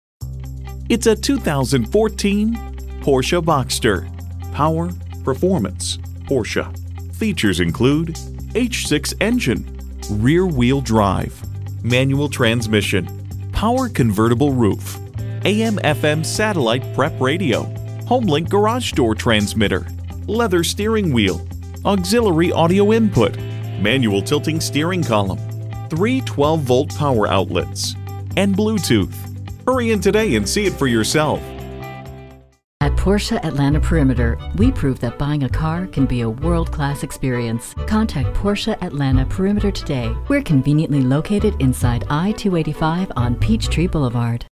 Used 2014 Porsche Boxster Atlanta, GA RP4095 - SOLD
Year: 2014
Make: Porsche
Model: Boxster
Trim: S
Engine: 3.4L H6 DI DOHC 24V ULEV II 315hp
Mileage: 131428
Stock #: RP4095
VIN: WP0CB2A87EK140095

Address:
4006 Carver Drive